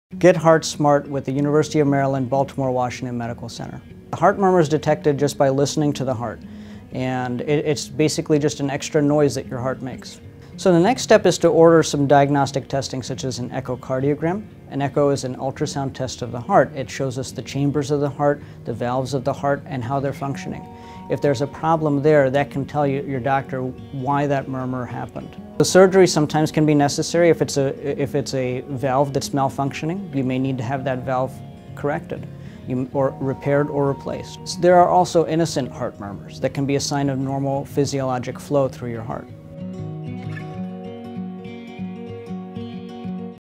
What is a Heart Murmur?
Listen to UM BWMC cardiologist Dr. Abid Fakhri describe what a heart murmur is and how it is treated.